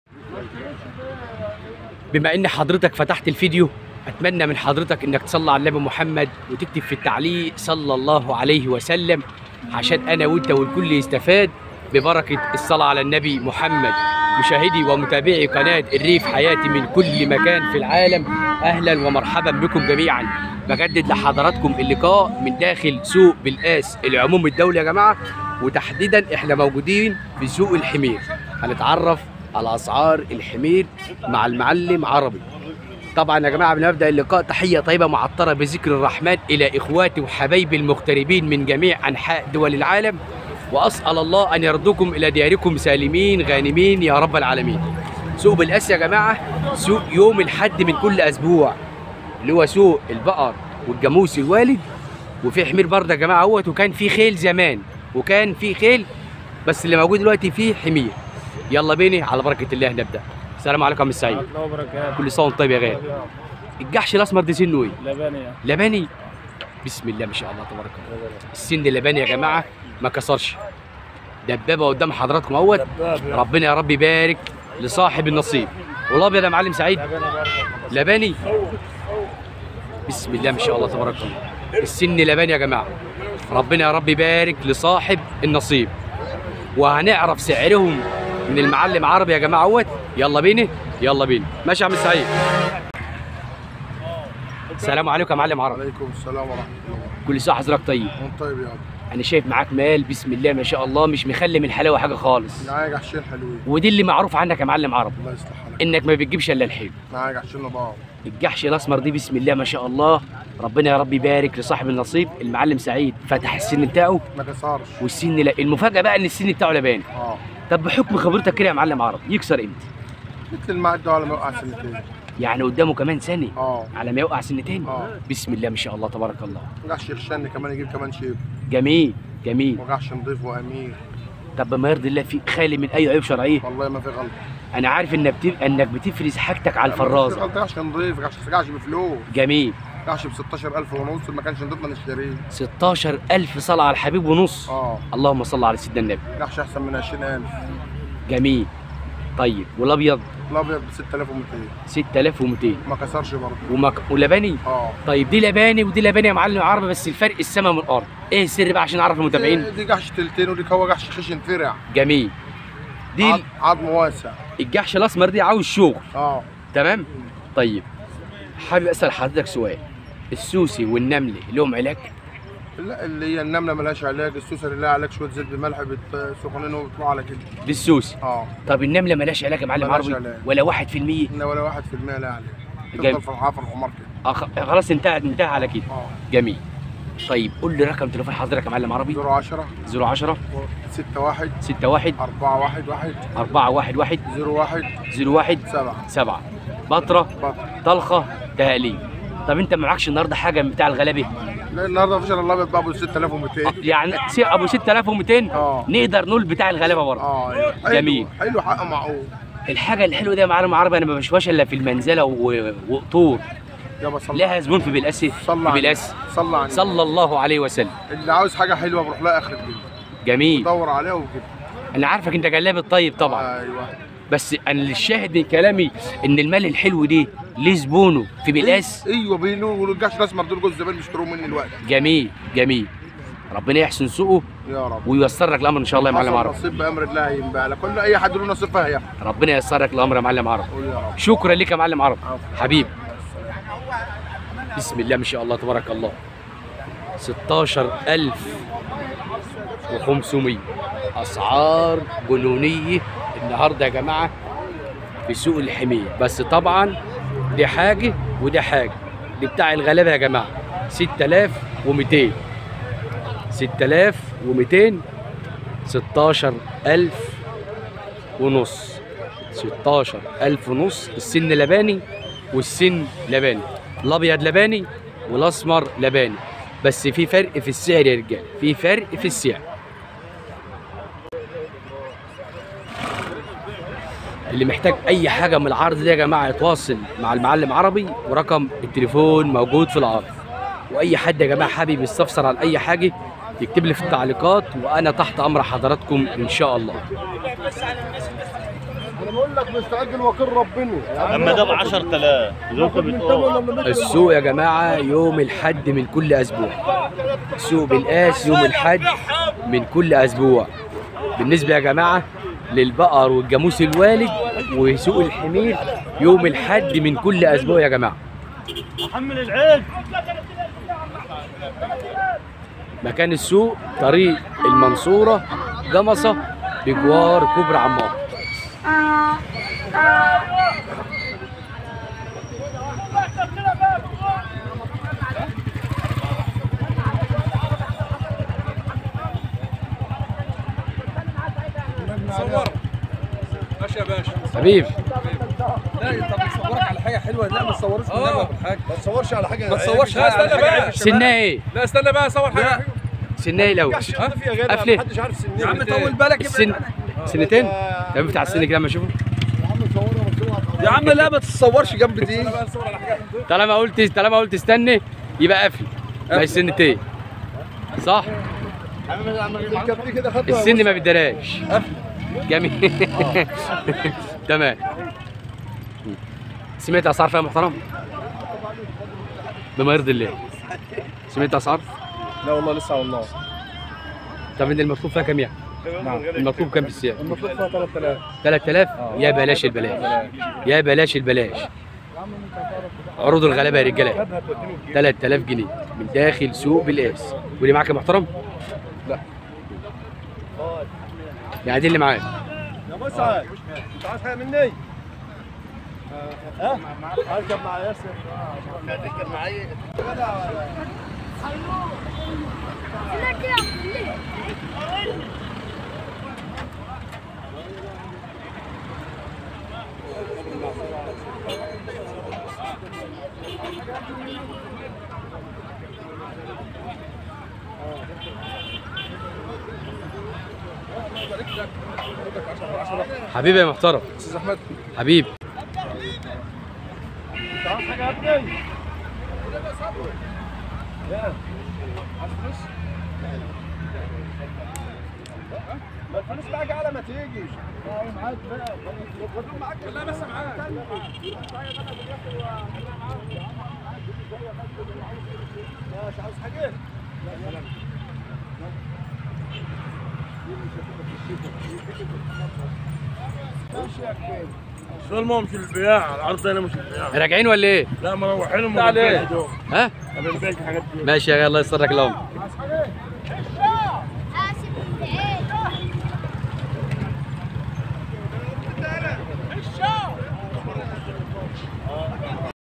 اسعار الحمير في سوق بلقاس اليوم مع المعلم العربي الصاوي بطره
اسعار الحمير اليوم مع المعلم العربي
سوق بلقاس اليوم
اسعار الحمير في سوق دسوق كفر الشيخ
كل مايخص الثروه الحيونيه  علي قناه الريف حياتي